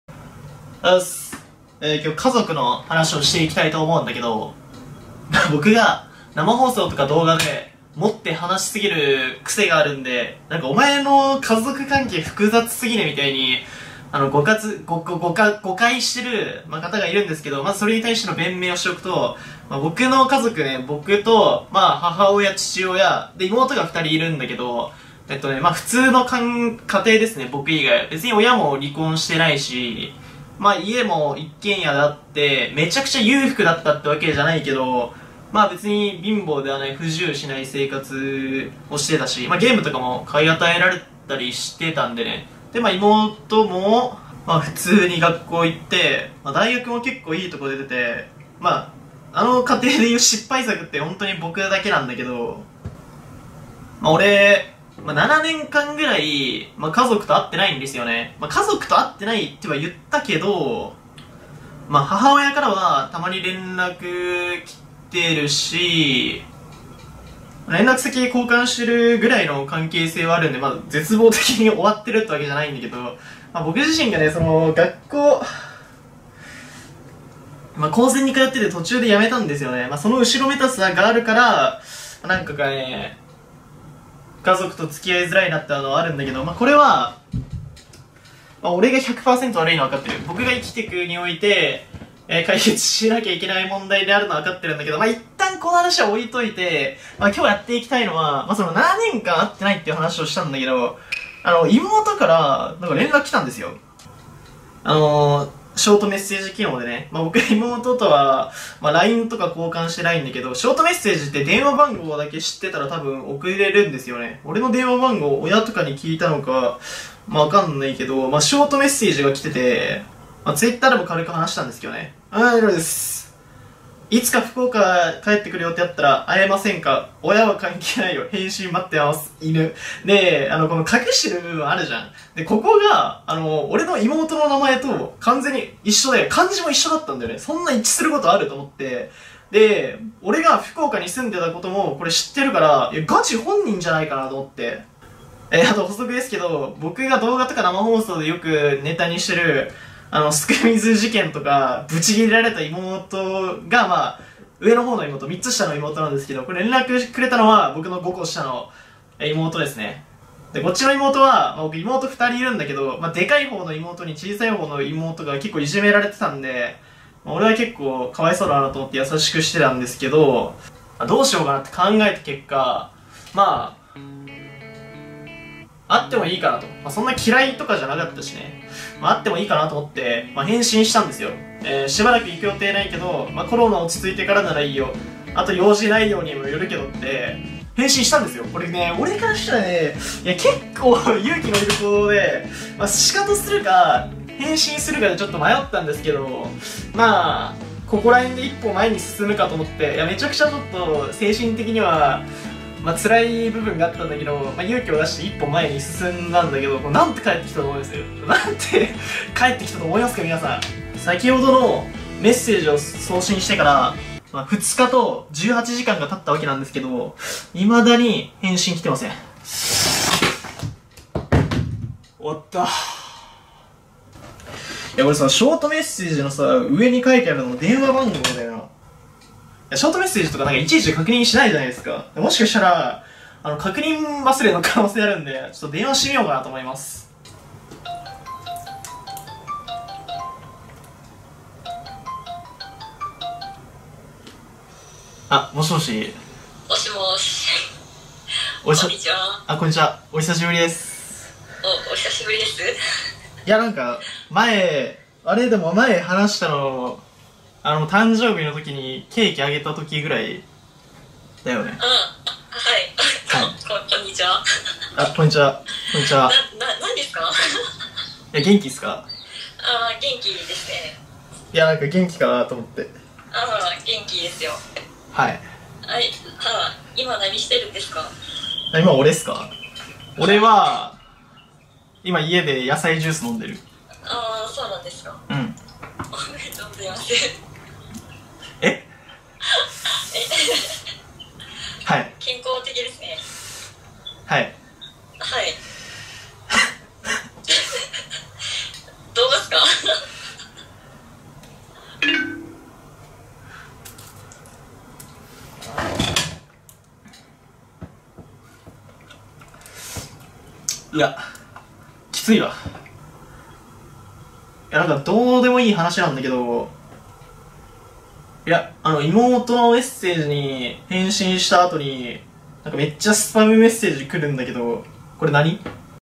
７年間連絡を取ってない「妹」に連絡してみた結果……
人間として成長するときがきた

このチャンネルはライバロリのサブチャンネルです。

【主に使用してるBGM＆効果音＆画像素材】
(騒音のない世界さん)